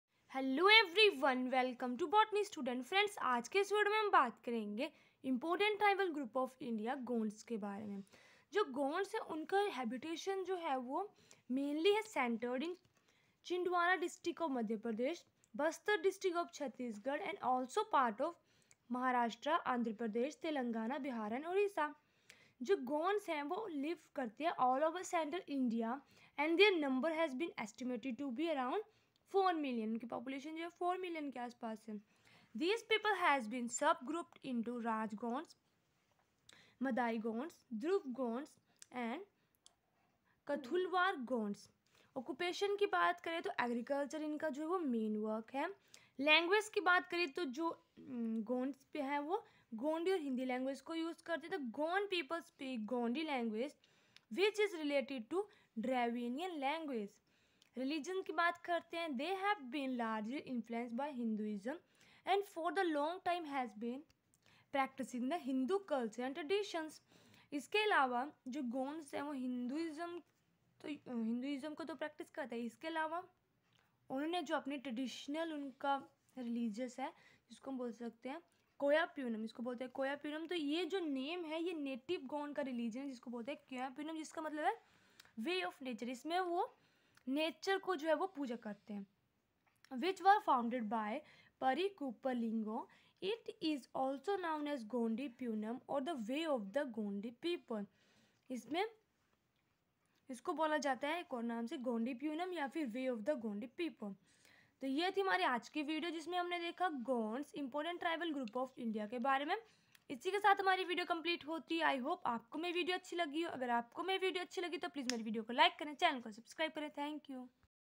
Gonds important tribal group of India their sub-group occupation,language,religion, koyapunem 🌿🌿🌿🌿🌿
Gonds important tribal group of india
 occupation culture
language
religion
population
koyapunem